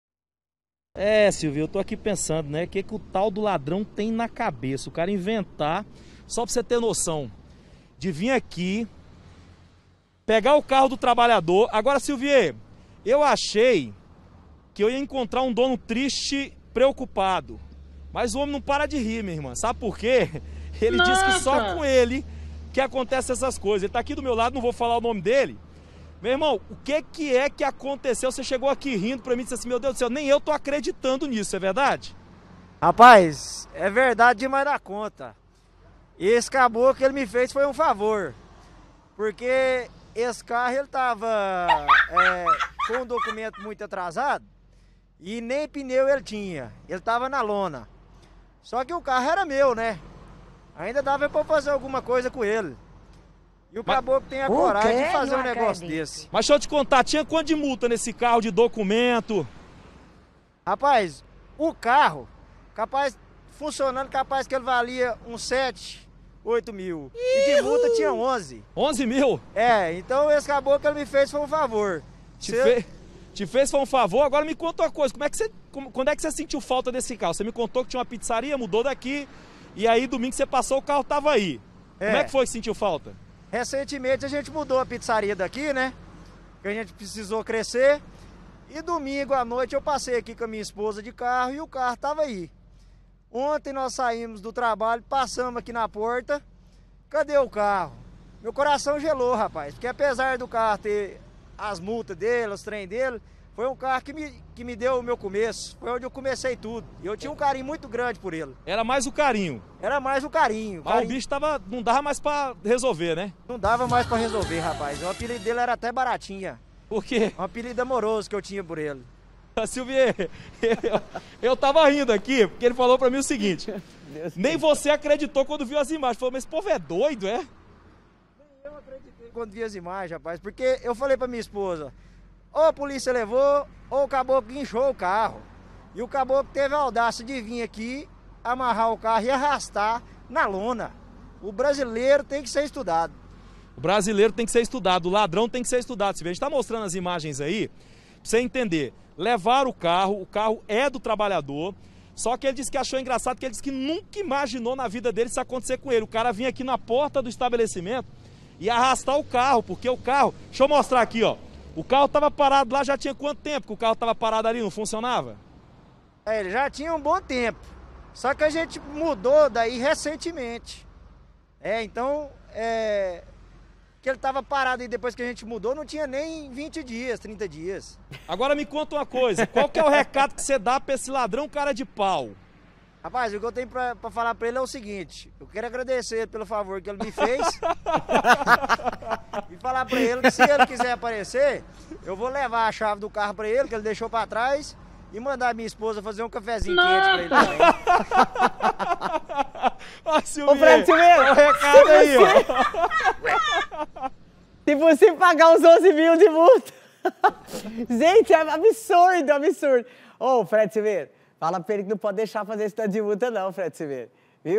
ESSE É NINJA: LADRÃO USA CORDA E SAI PUXANDO CARRO EM AVENIDA MOVIMENTADA
VAMOS FALAR NOVAMENTE COM O FRED SILVEIRA SOBRE O LADRÃO FOLGADO QUE ROUBOU O CARRO USANDO UMA CORDA.